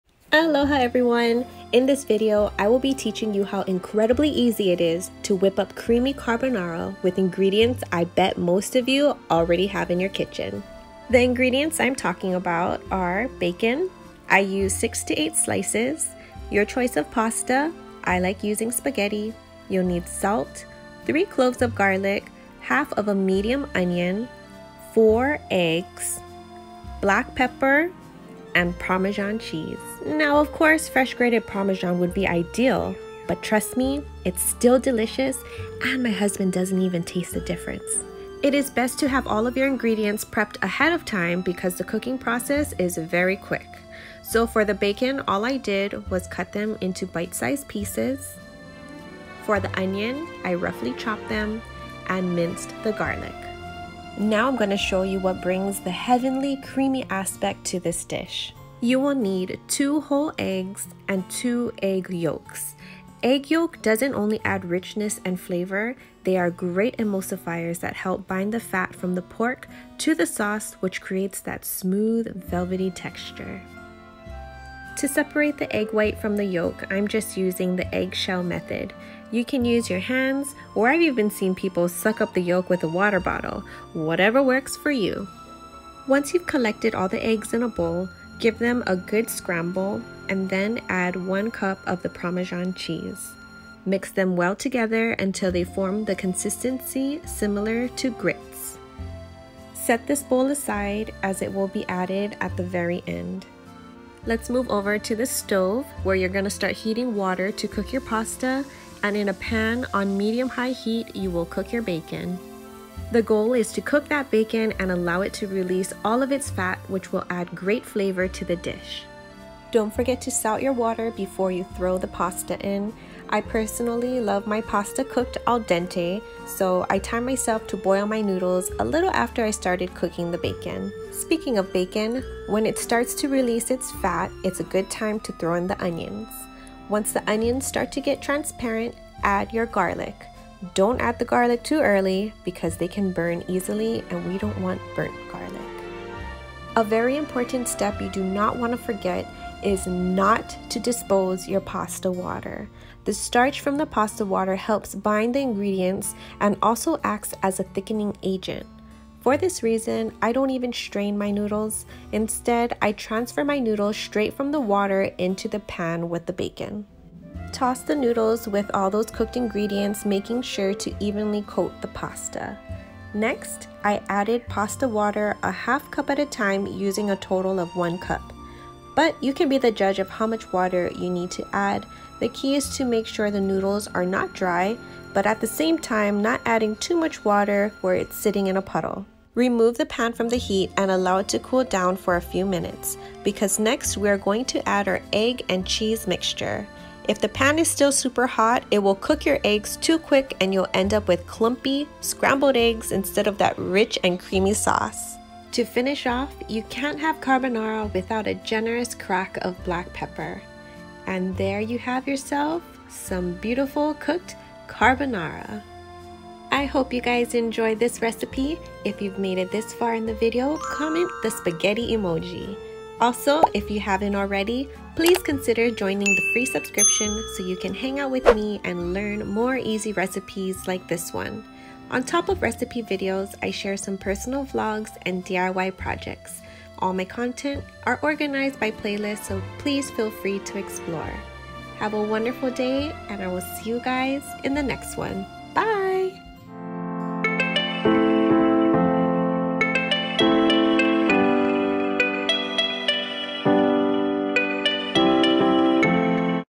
8 ingredient CARBONARA RECIPE | Sally Funakoshi
We love PASTA!  Here is one of my go to recipes to satisfy those cravings!

Ingredients
Bacon 6-8 slices
Pasta of choice
Salt
3 cloves garlic
1/2 a medium onion
4 eggs (2 whole, 2 yolks only)
Black pepper
1 cup parmesan cheese

Current upload schedule:
Tuesday & Friday
6am (Hawai'i Time)